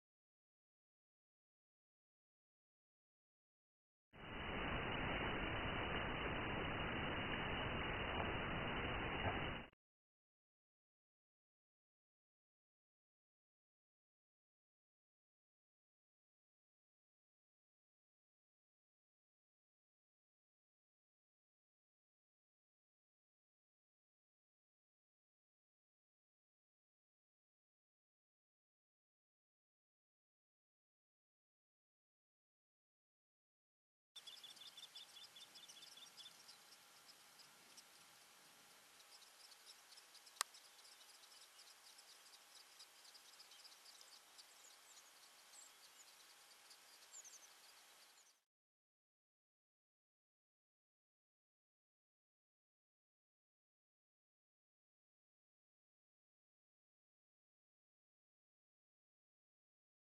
Premiers putois d'Amérique nés dans la nature au parc national des Prairies
Images vidéo prises dans le parc national du Canada des Prairies la nuit du 24 juillet 2010, présentant trois jeunes putois d'Amérique jouant à la lueur de la lune sous le regard protecteur de leur mère. Ces trois jeunes putois sont les premiers nés en liberté au Canada à avoir été observés depuis plus de 70 ans.